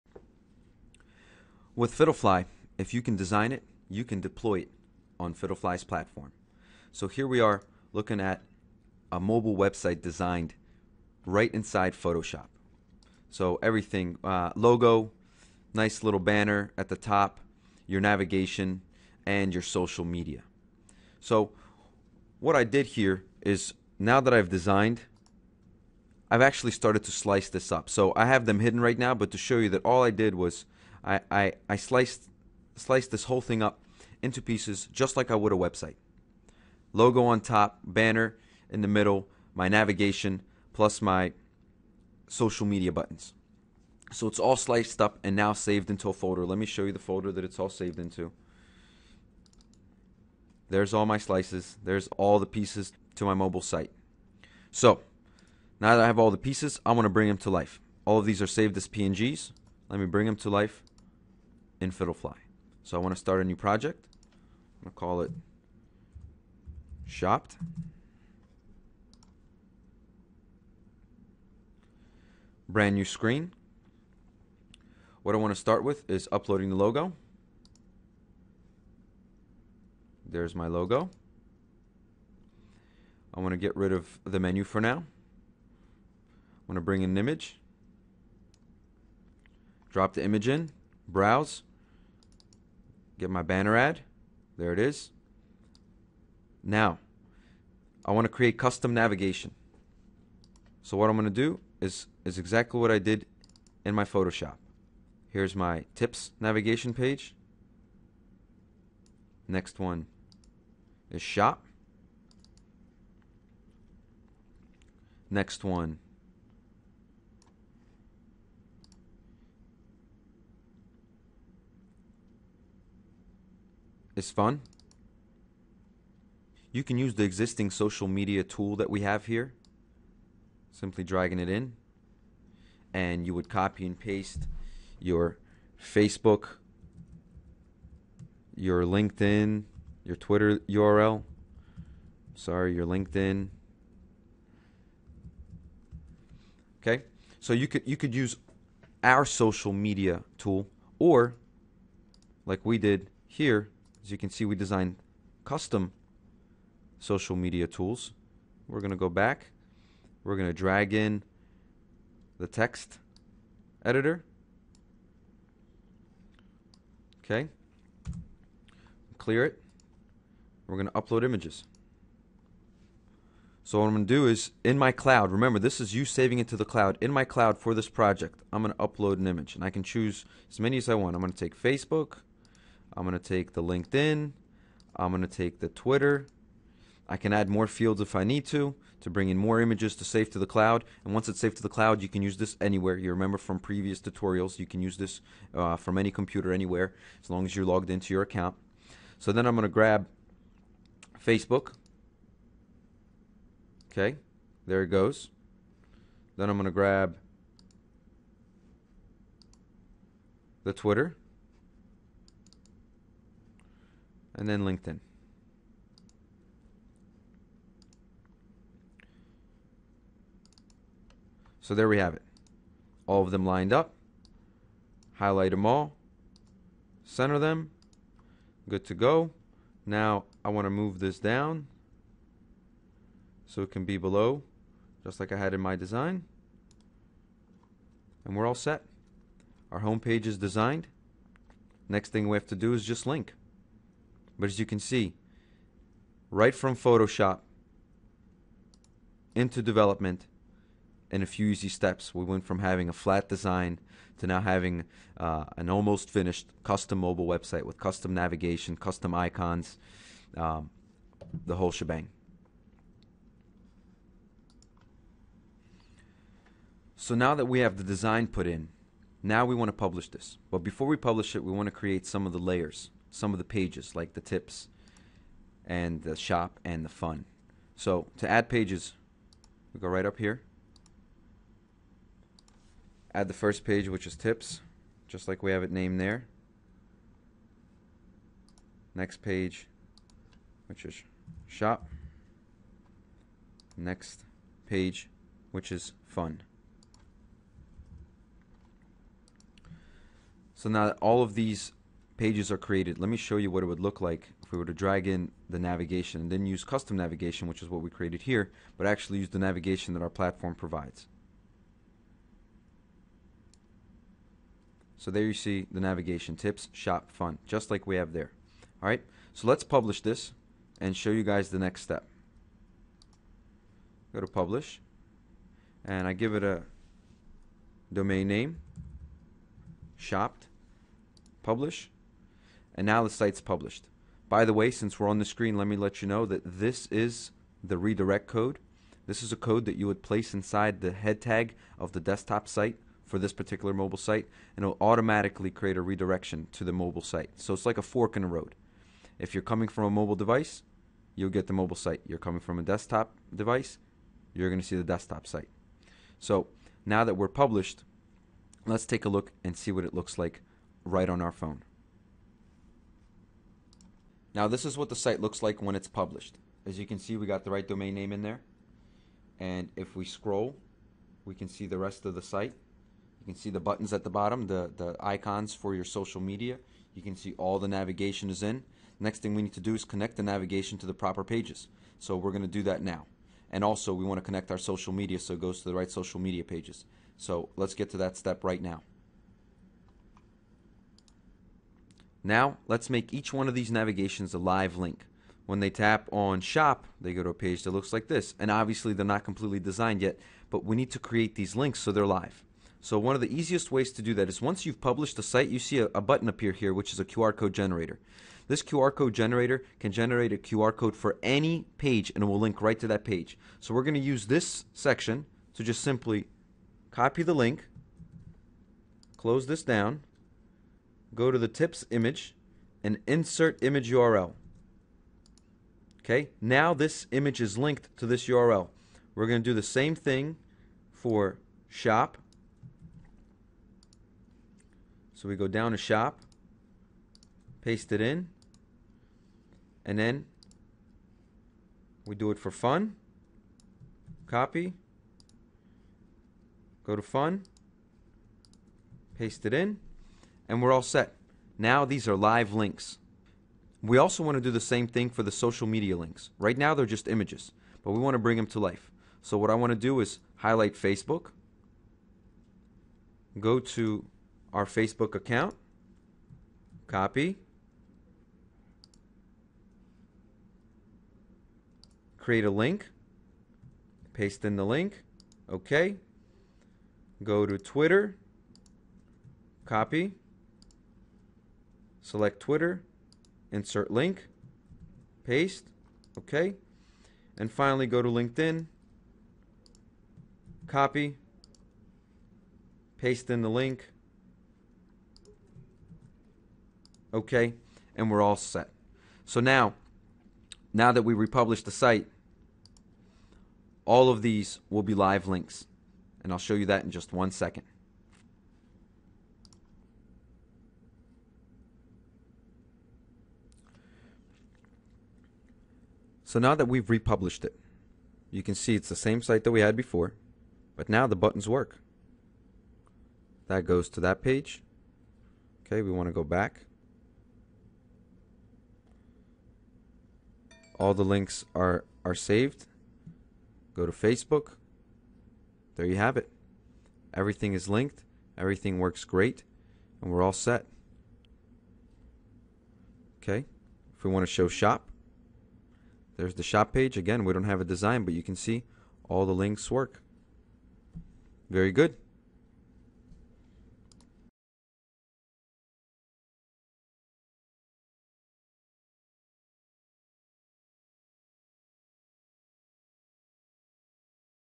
From Photoshop to Mobile Web in Minutes
Fiddlefly - Mobile web building platform lets you go from photoshop to development in minutes. This video will blow your mind. You can try it yourself for free